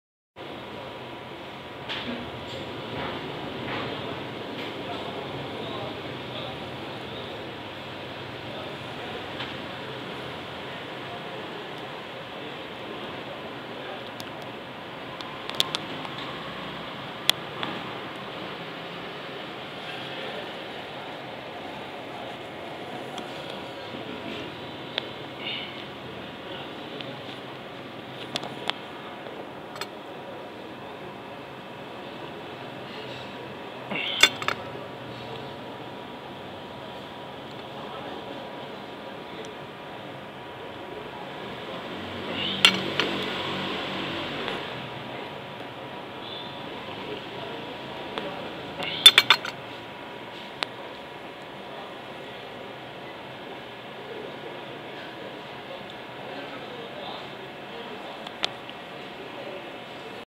Having coffee in Hangout Cafe' & Bistro...
Today is Sunday 9 August 2015...This morning I had coffee in Hangout Cafe' & Bistro...I recorded this video while having coffee...Subu haana Allah...Glory be to Allah...